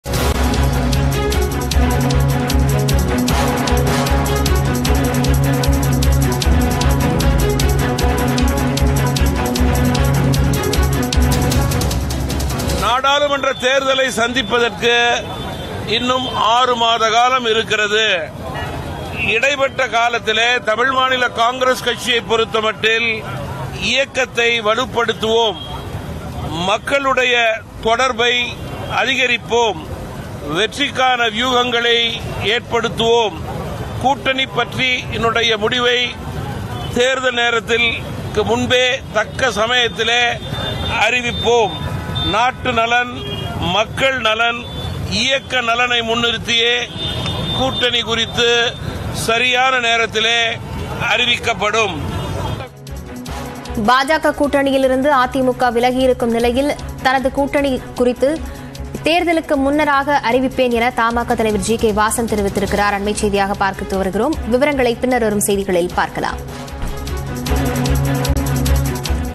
BREAKING | தேர்தலுக்கு முன்பு கூட்டணி குறித்து அறிவிப்பேன் - ஜி.கே.வாசன் | GK Vasan | NDA | ADMK
தேர்தலுக்கு முன்பு கூட்டணி குறித்து அறிவிப்பேன் - ஜி.கே.வாசன். செய்திகளை துல்லியமாகவும் துரிதமாகவும் வழங்கும் தமிழகத்தின் முதன்மை செய்தி தொலைக்காட்சி உங்கள் நியூஸ்18 தமிழ்நாடு.